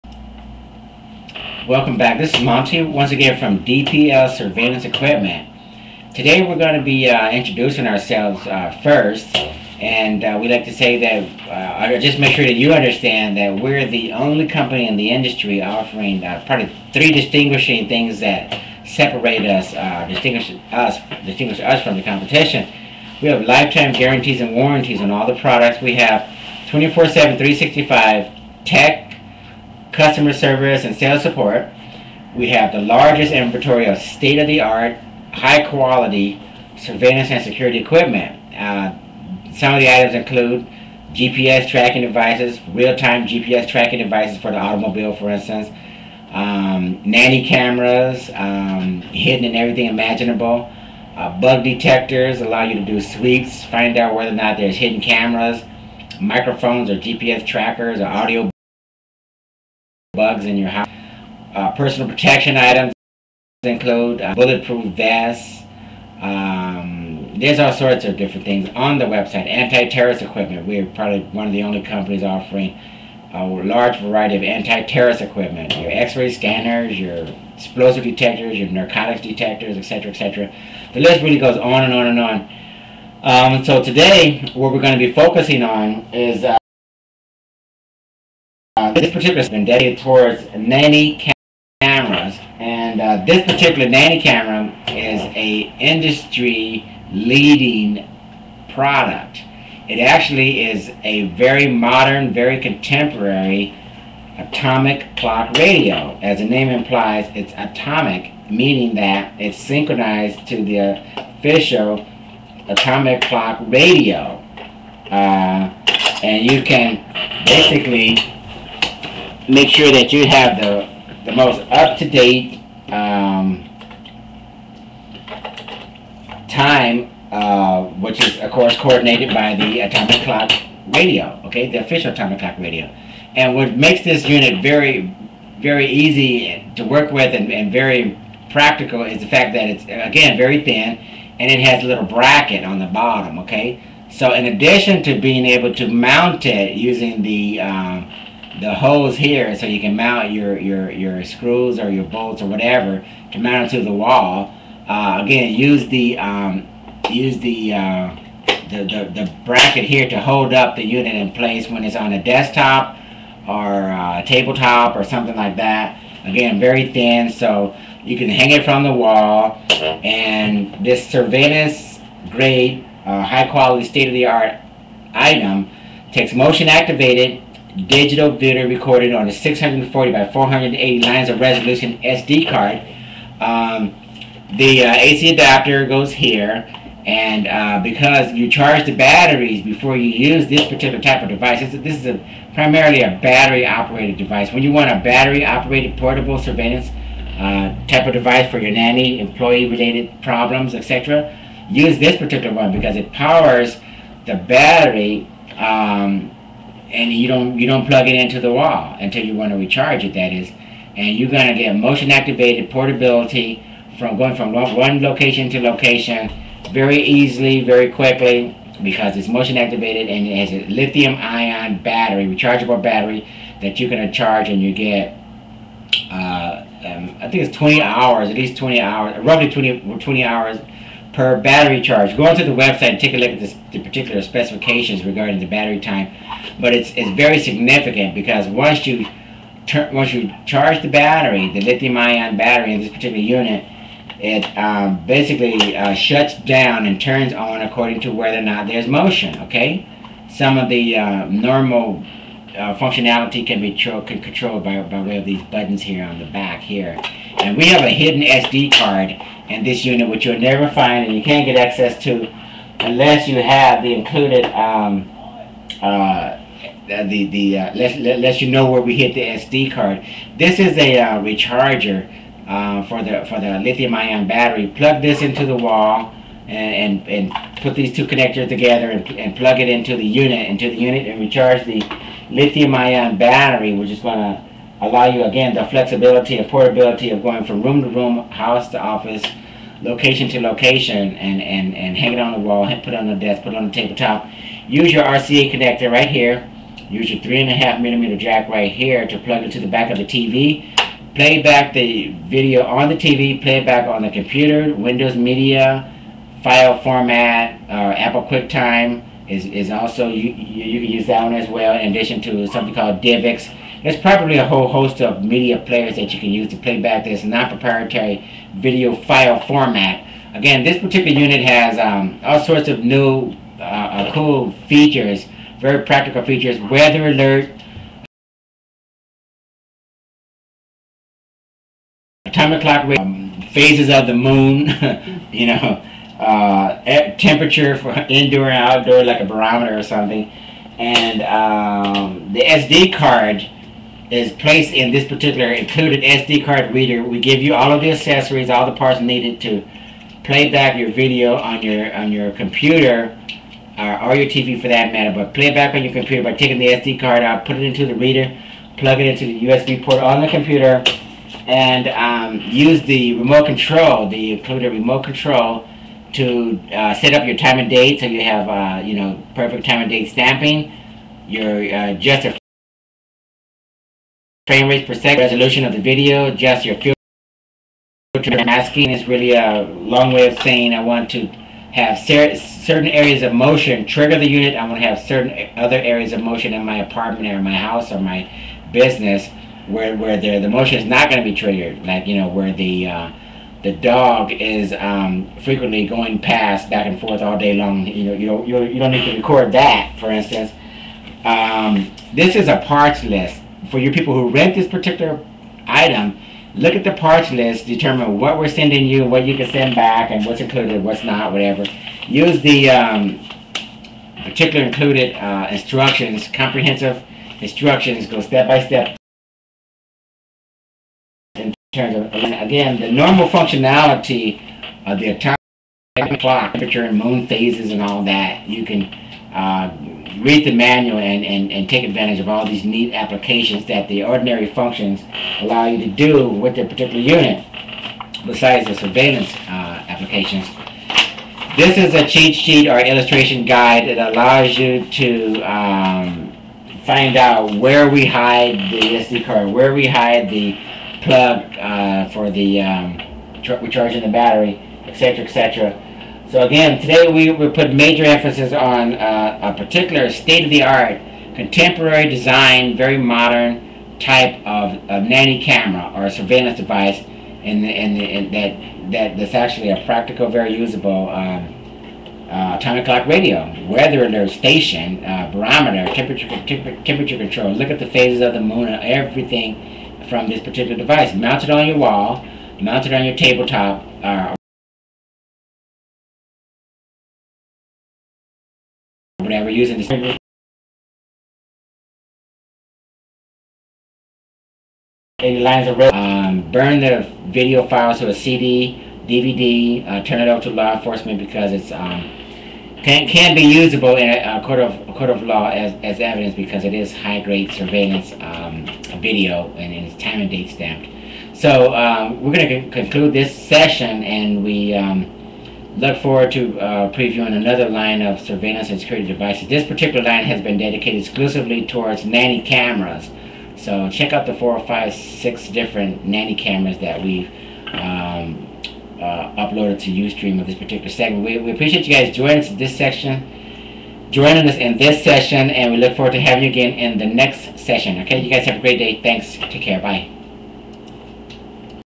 ATOMIC WALL / DESKCLOCK HIDDEN CAMERA w/ BUILT-IN BATTERY-POWERED (Buy/Rent/Layaway)
No more power cords required! Our totally portable Atomic Wall / Desk Clock Spy Camera / DVR (with Battery Powered, PIR Motion Activation Technology)can quickly be placed in any indoor location without the worry of AC outlets or extension cords. PIR Motion Activation technology, Li-ion battery powered, Atomic Wall/Desk Clock DVR. Utilizing our copyrighted Micro PIR technology, this unit records 20 hours of actual video and last one full year in "standby mode" on a single charge.

(Buy/Rent/Layaway)